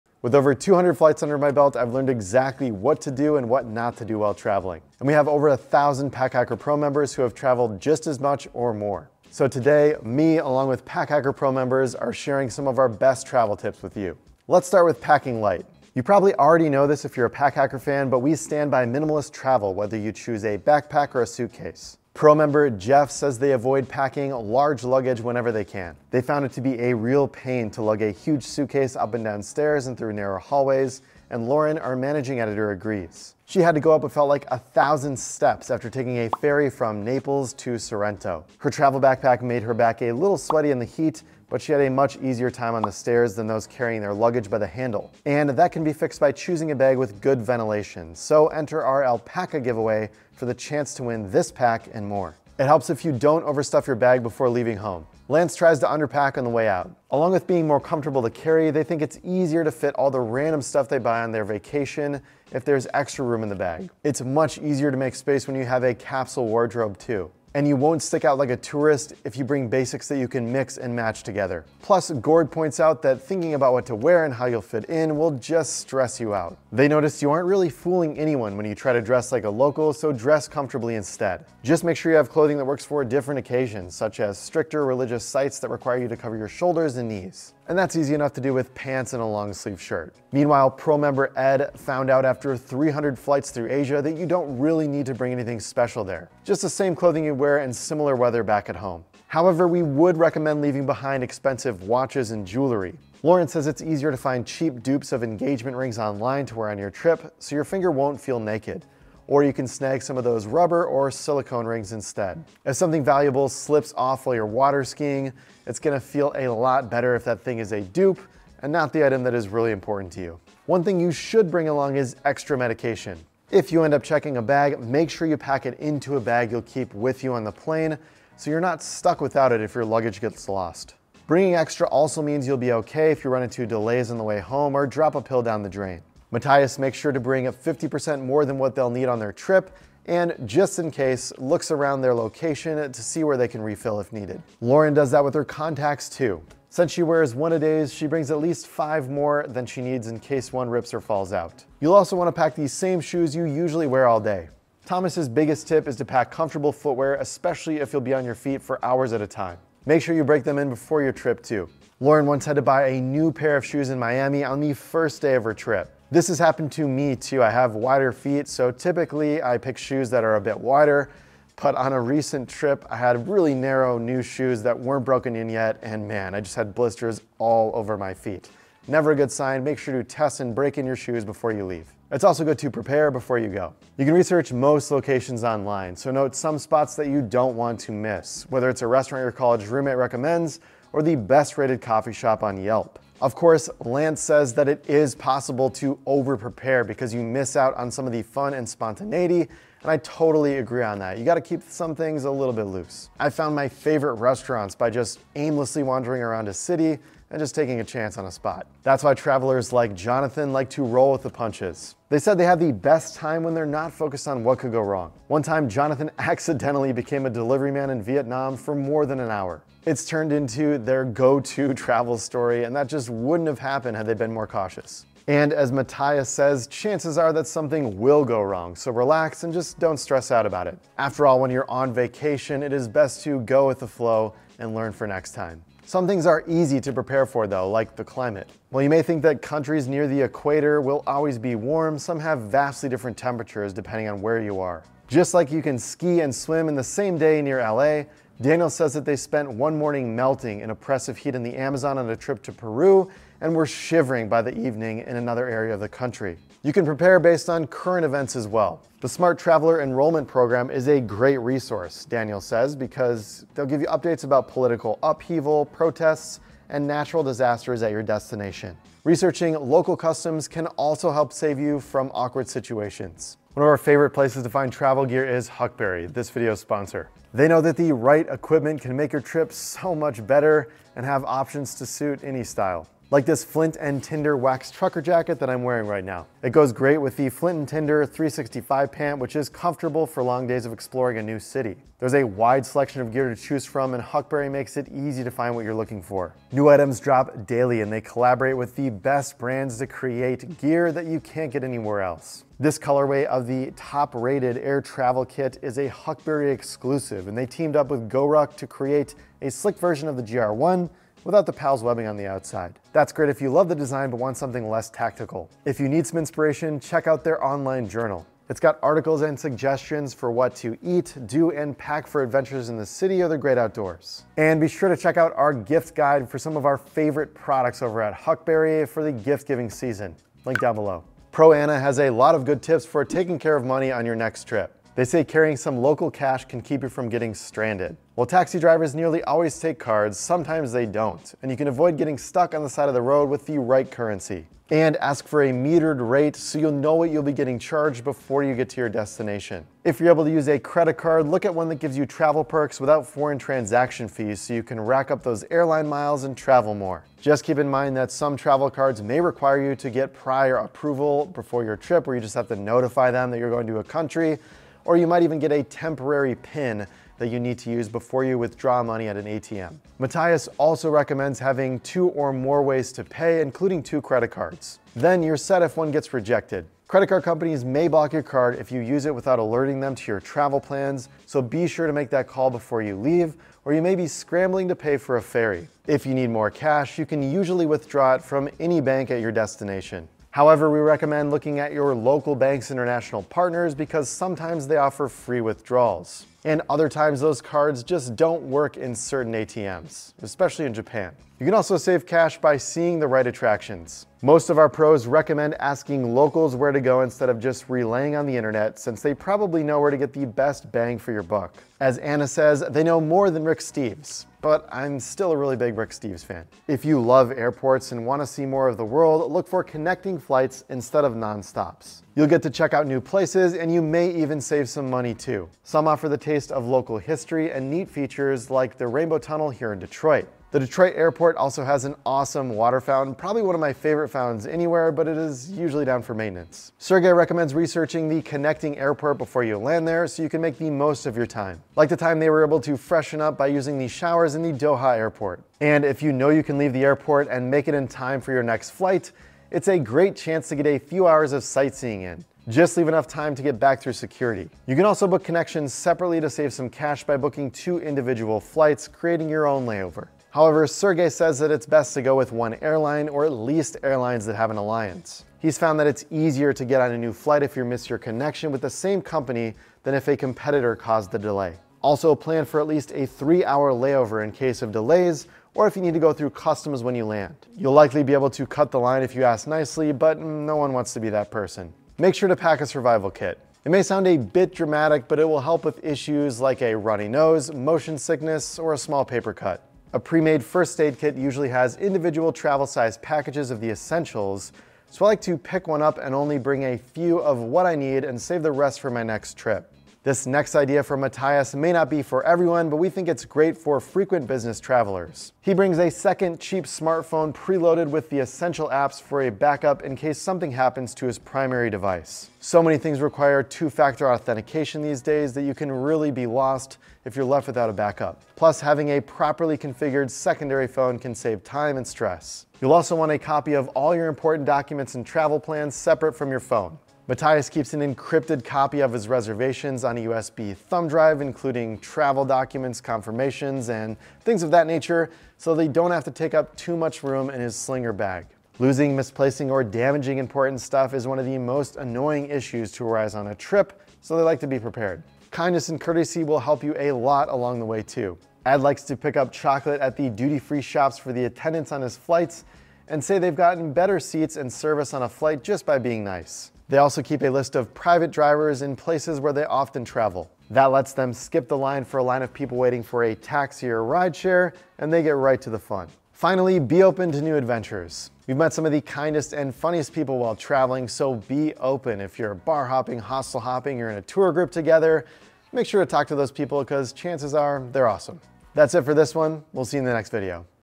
Travel Experts Share Their Best 20 Travel Tips
Our Pack Hacker Pros have some great travel advice for you! Get the best travel tips before your next trip. Check out our Gift Guide for a curated list of our favorite products on Huckberry

0:00 - Intro
0:15 - Packing And Preparation
5:32 - Money Matters
8:02 - Bring A Backup

Do you know what you should bring backups for and what gear you can leave behind when you travel? Or are you wondering if packing minimally is worth it? With thousands of collective flights and trips under their belts, we and our Pros have some of the best travel tips for you.